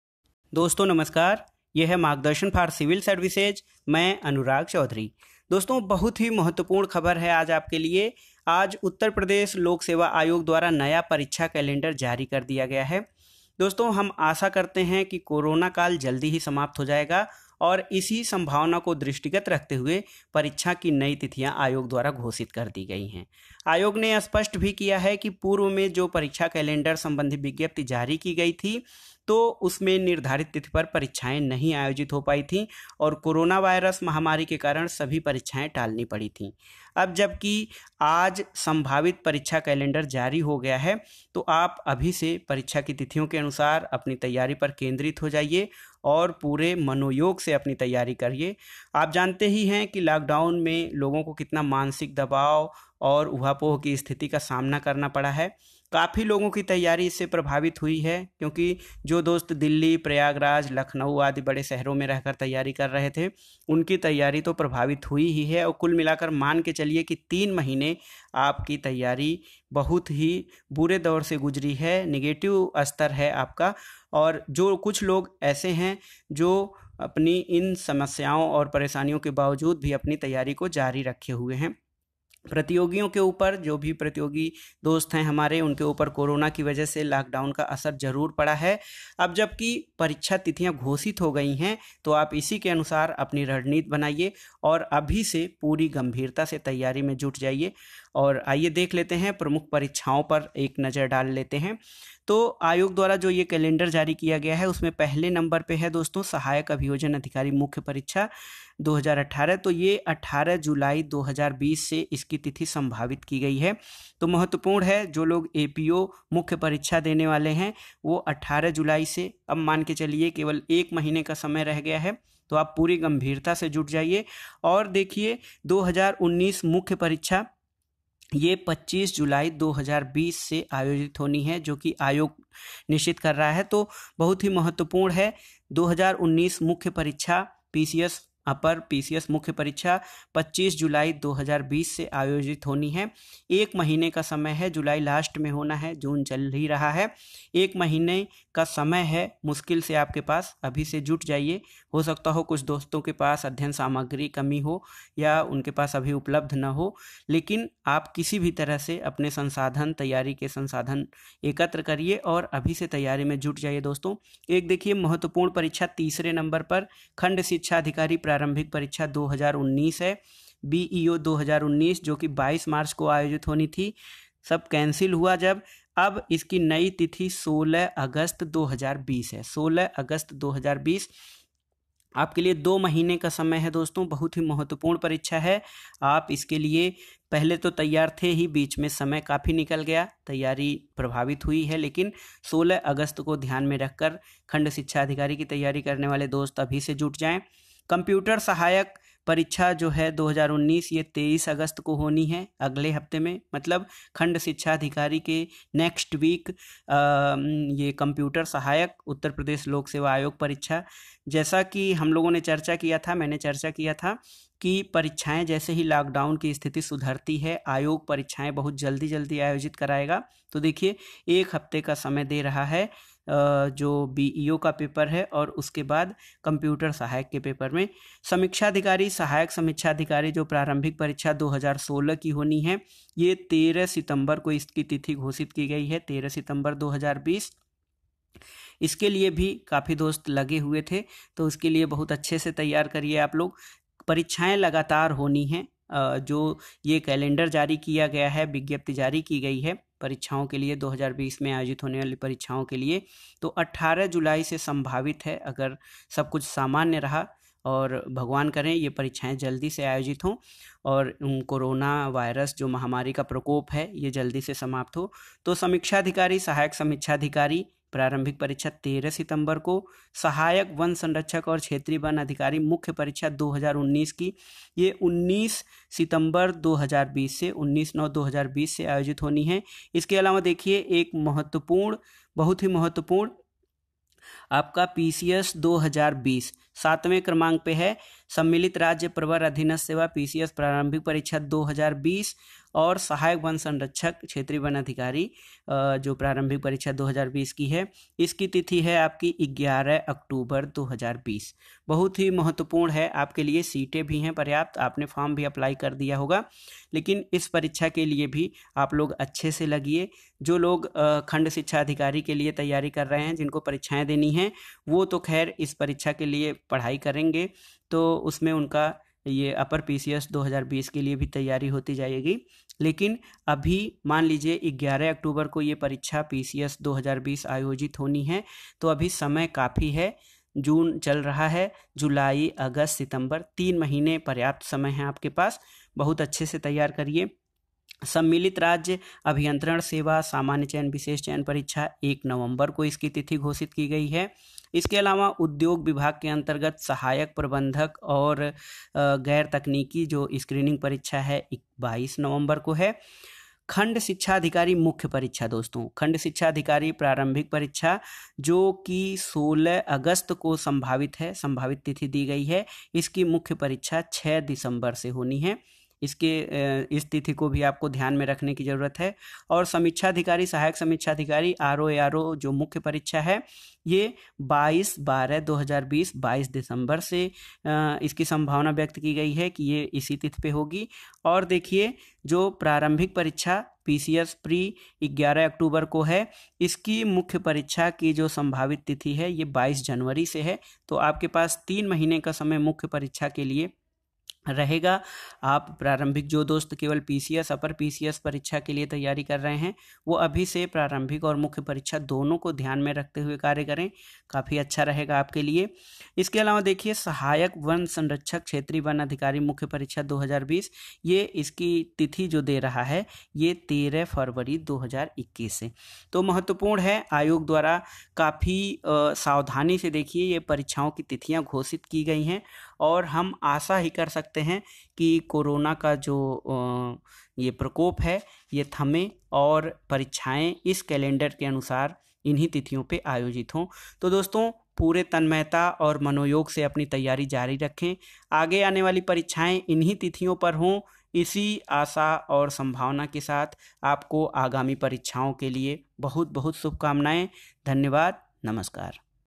UPPSC परीक्षा कैलेंडर जारी। UPPCS exam calendar 2020।
उत्तर प्रदेश लोक सेवा आयोग द्वारा वर्ष 2020 के लिए नया परीक्षा कैलेंडर जारी...
UPPSC Exam Calendar 2020,

Uppsc prayagraj exam calendar,
uppsc exam date declared,